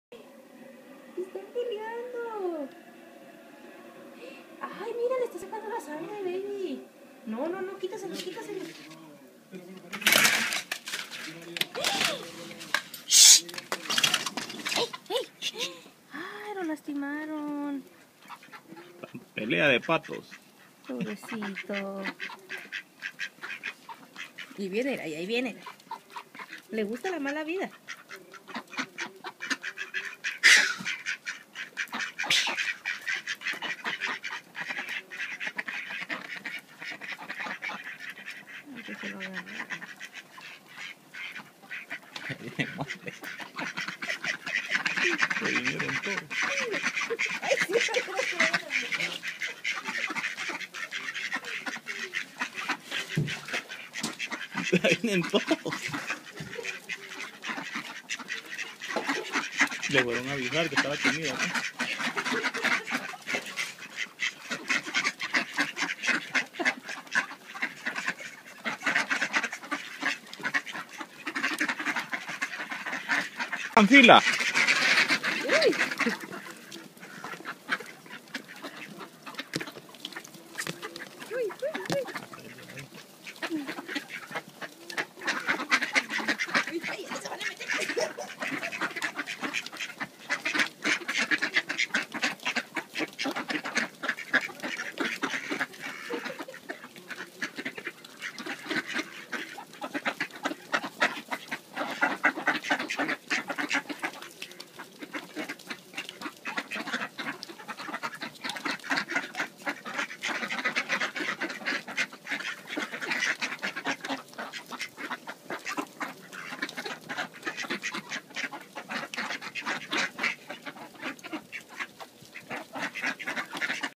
Pelea de Patos con hambre
Como hay mucha nieve andan hambrientos